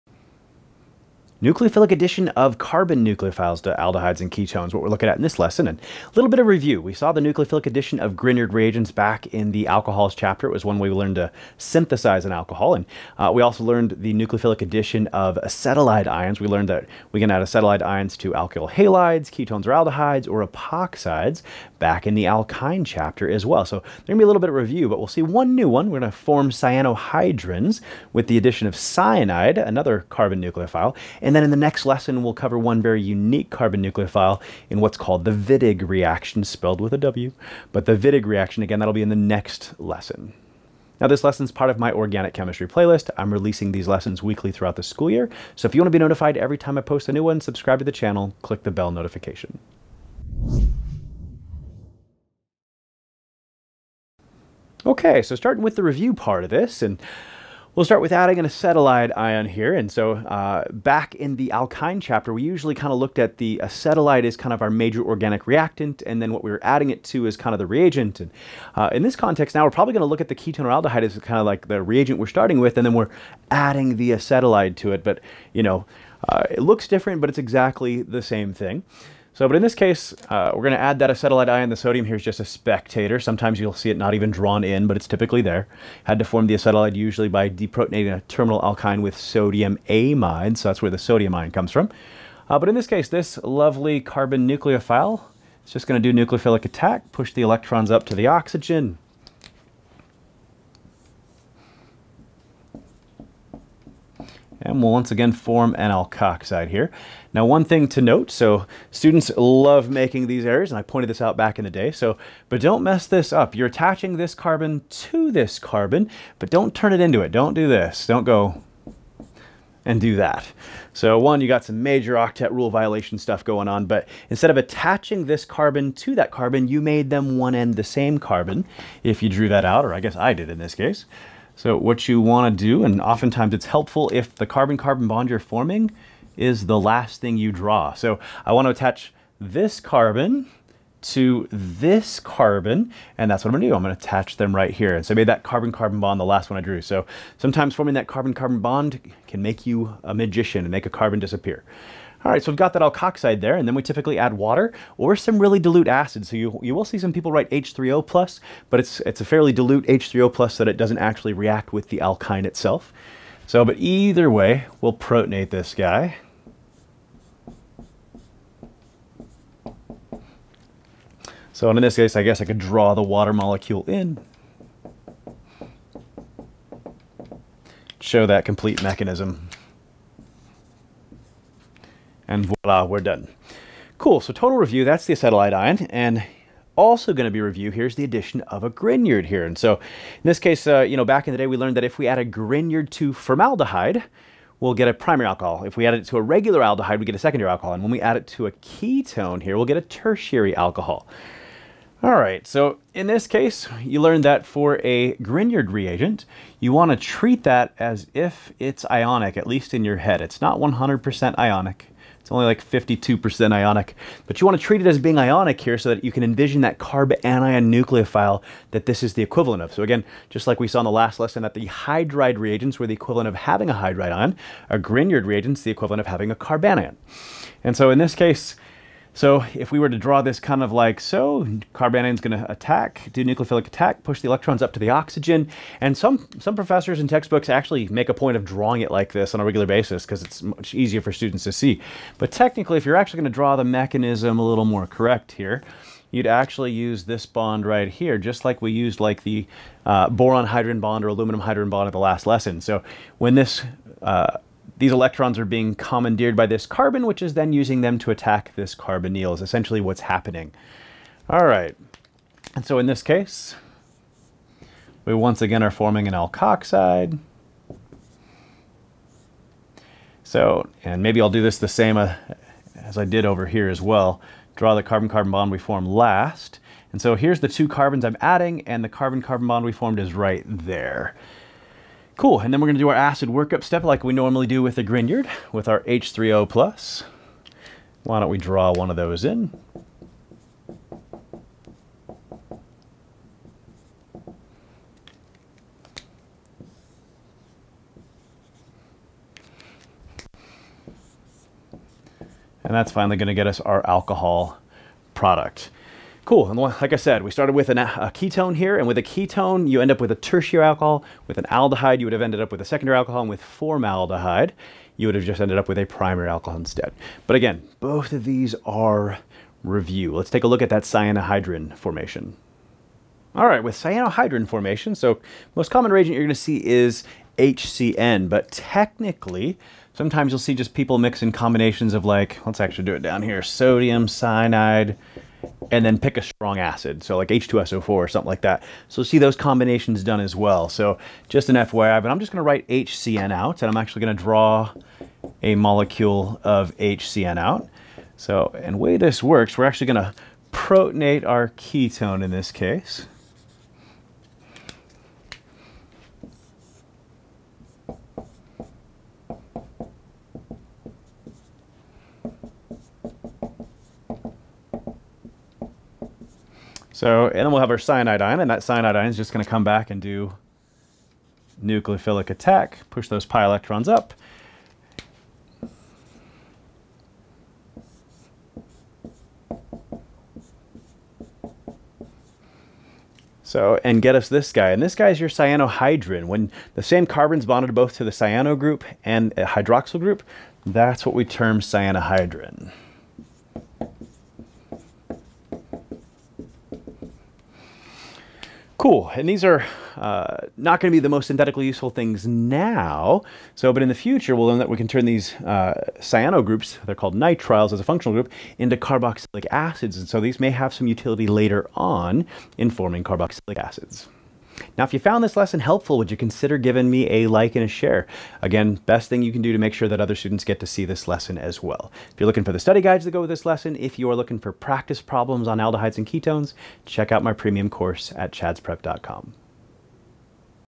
19.7a Addition of Grignard Reagents (and other Carbon Nucleophiles)
In this lesson Chad presents the nucleophilic addition of various carbon nucleophiles to aldehydes and ketones.  This includes a review of the addition of acetylides and grignard reagents to ketones and aldehydes to form alcohols as well as the addition of HCN to ketones and aldehydes to form a cynohydrin.  The acetylide addition mechanism, grignard reaction mechanism, and cyanohydrin formation mechanism are all presented as well.

00:00 Lesson Introduction
00:54 Acetylide Addition to Aldehydes and Ketones
03:06 Grignard Reaction with Aldehydes and Ketones
05:22 Cyanohydrin Formation